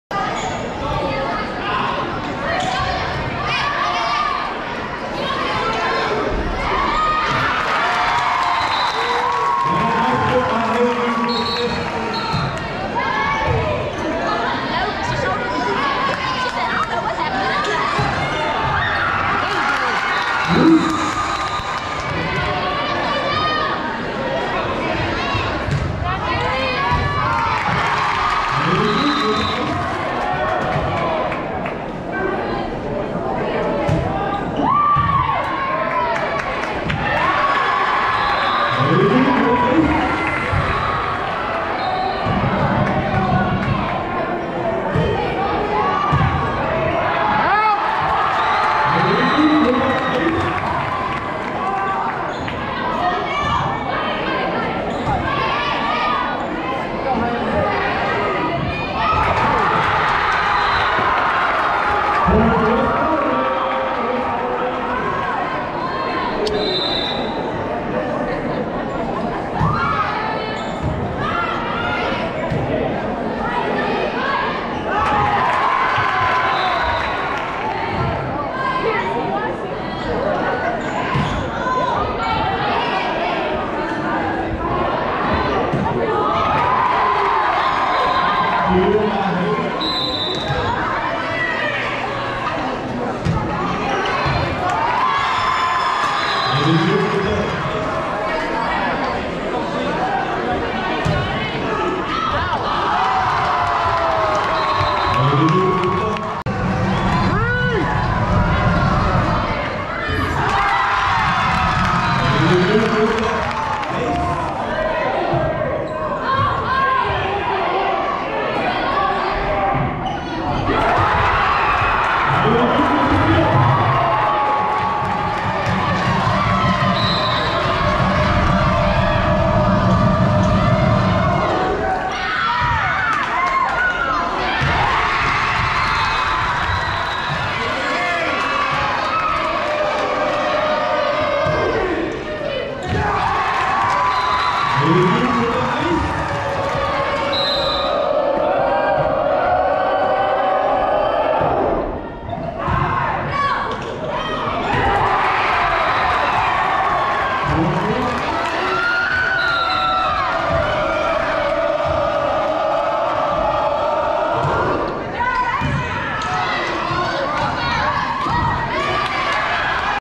Hailey Duque Class of 2024 Volleyball Recruit WLHS vs Clarkdale 9/13/21
Hailey Duque,
Current Sophomore, WLHS Varsity, OH/RH, #15
Graduates 2024
Southern Select Volleyball Club